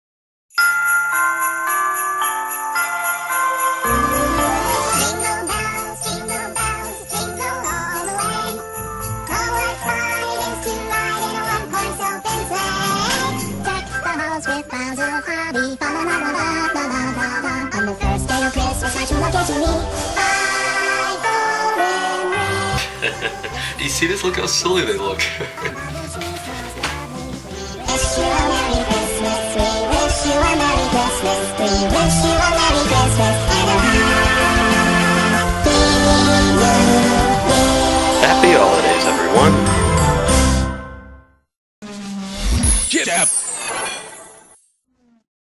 X-mas Greeting, 2009
Happy Holidays from everyone in Chocobro Cinemas!

(Made with the online program known as "Elf Youself," powered by JibJab.)